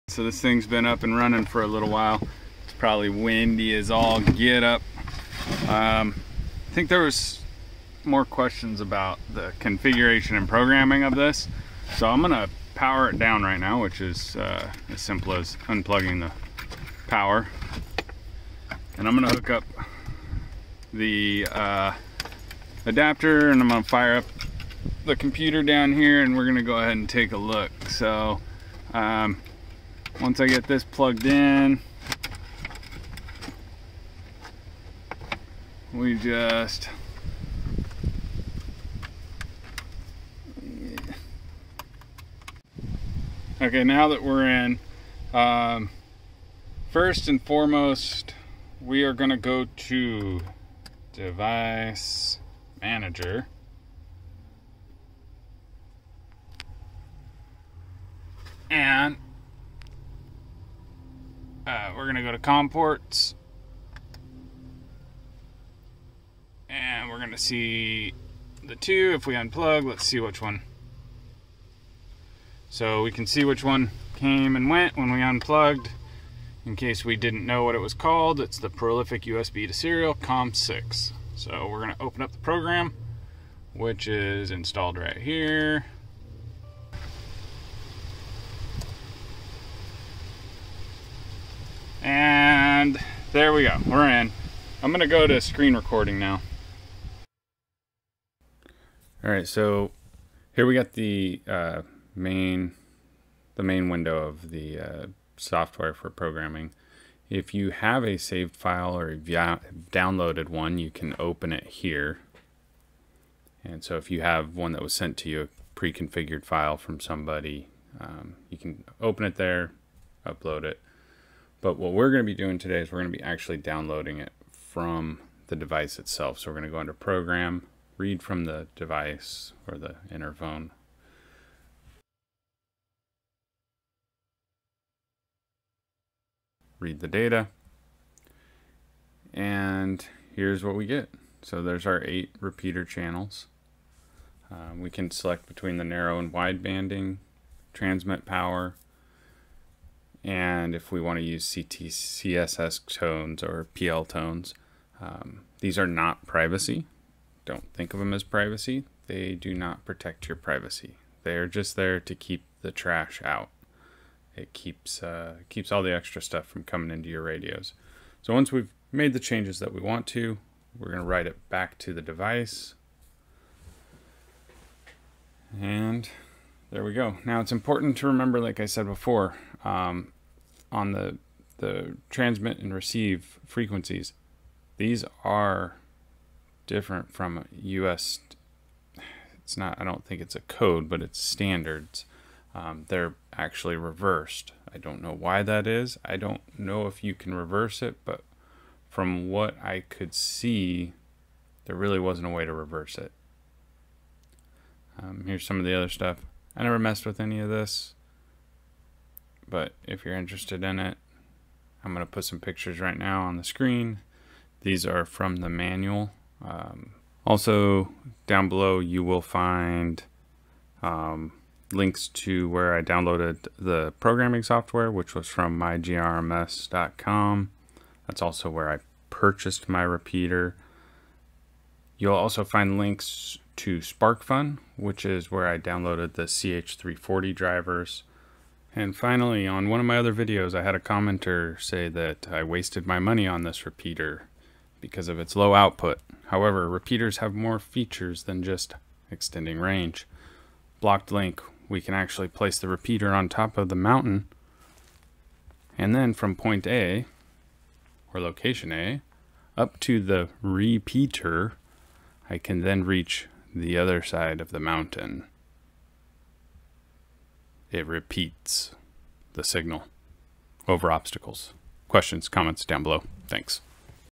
Retevis RT97S programing screen, software, and Download and upload configurations.
This is just a quick look at getting to the programming and setup screen of the Retevis RT97S. If you need advise on the actual settings, there are far better tutorials out there. Also, as an update, yes the repeater has been just fine. Still worth the money. Dont use it as much as I thought I would but it is a nice to have.

if you need the CH340 drivers, because the prolific usb driver from mygmrs.com doesn't work or using aftermarket cables

I used the Ubiquiti antenna link tool screen shots from my 5Ghz antenna links. They may be at slightly different heights, but the GMRS radios and repeater at 460 Mhz don't mind as much about slight blocking or trees.

Also, I have had no issues with the repeater antenna next to the Ubiquiti 5Ghz or 900Mhz radios.